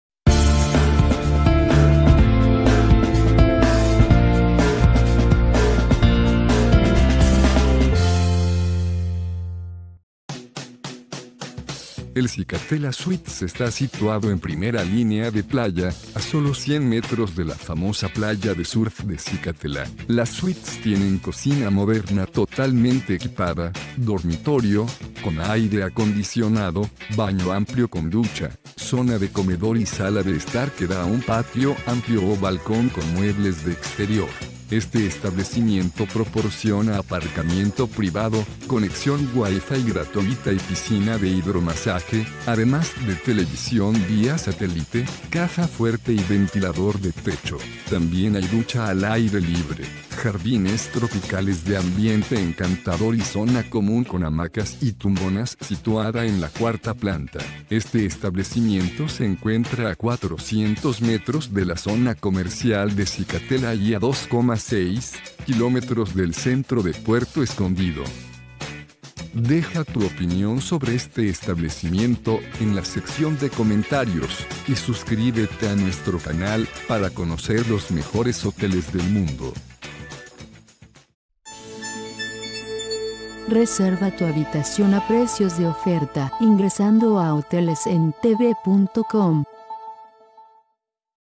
Zicatela Suites, Apartamentos en Puerto Escondido
Zicatela Suites ubicado en Calle del Morro s/n, Colonia Santa Maria, presenta Apartamentos con jardín, terraza y habitaciones no fumadores. Su amable personal te asistirá en cualquier problema que puedas tener durante tu estancia. El check in comienza a las 14:00 y el check out  finaliza a las 18:00.